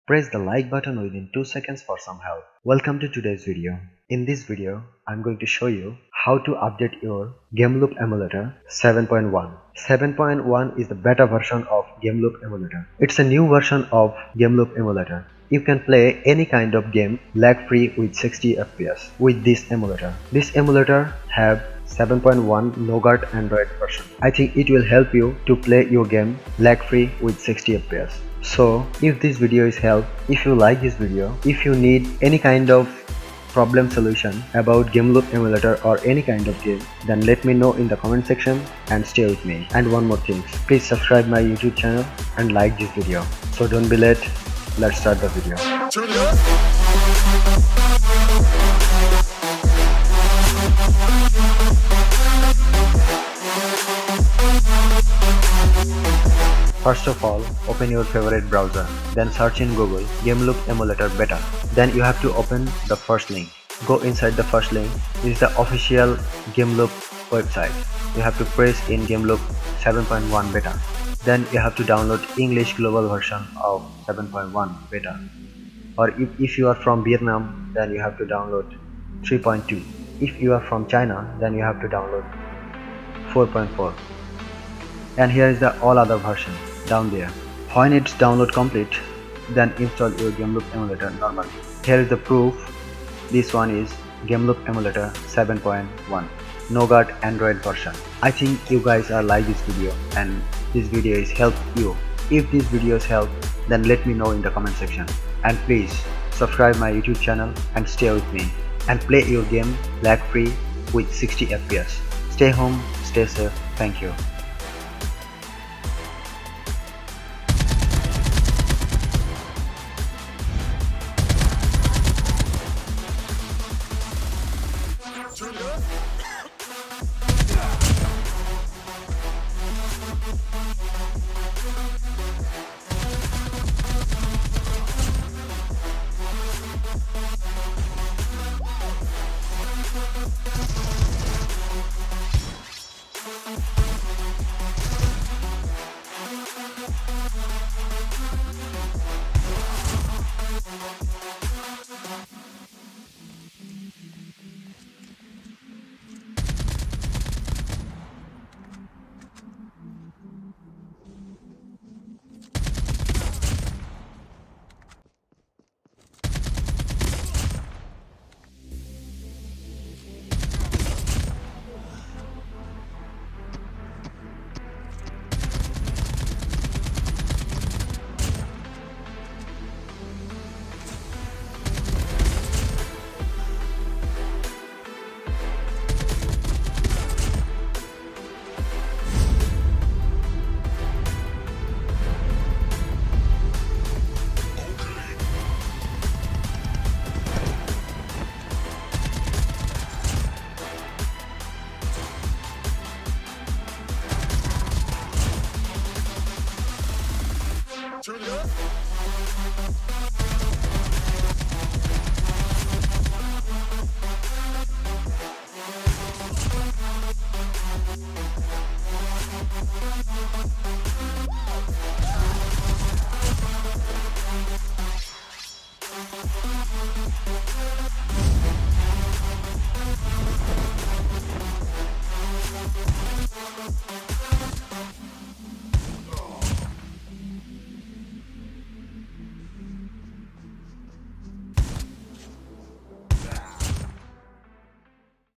HOW TO DOWNLOAD AND INSTALL GAMELOOP EMULATOR 7.1 BETA //GAMELOOP 7.1 BETA
In this video I am going to show you How to donload and install Gameloop emulator 7.1 beta.
If you install 7.1 gameloop I thing you can play any kind of mobile game lag free With 60 FPS. So Please try it i think it will give u best experience.
Thanks.

Timestamps:
0:00 - Topic
0:45 - Intro
0:55 - Main
1:53 - Gameplay

Title:- HOW TO DOWNLOAD AND INSTALL GAMELOOP EMULATOR 7.1 BETA //GAMELOOP 7.1 BETA